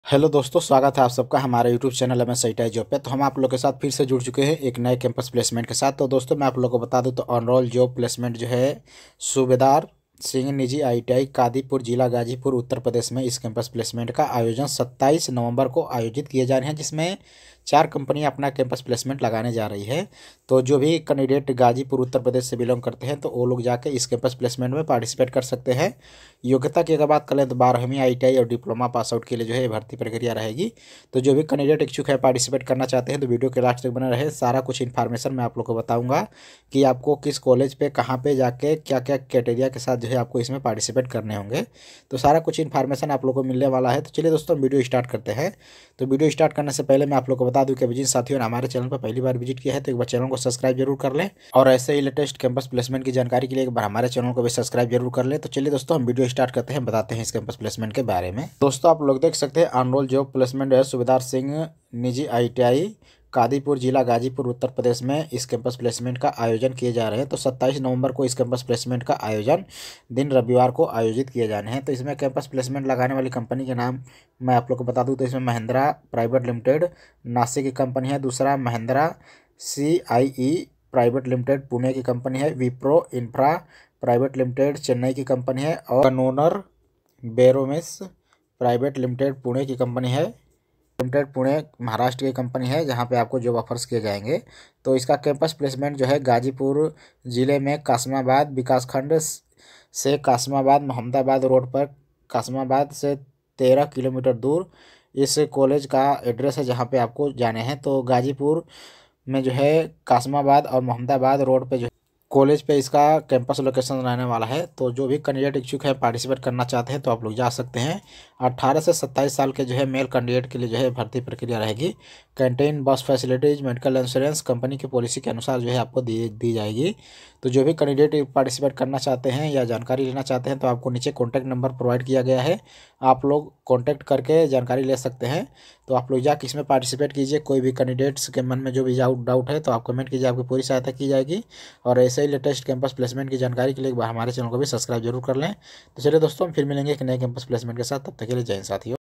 4 कम्पनीयो का महारोजगार मेला गाजीपुर उत्तर प्रदेश 27 नवम्बर 2022|12 iti Deploma Pass
4 कम्पनीयो का महारोजगार मेला गाजीपुर उत्तर प्रदेश 27 नवम्बर 2022|12 iti Deploma Pass @asitijob3396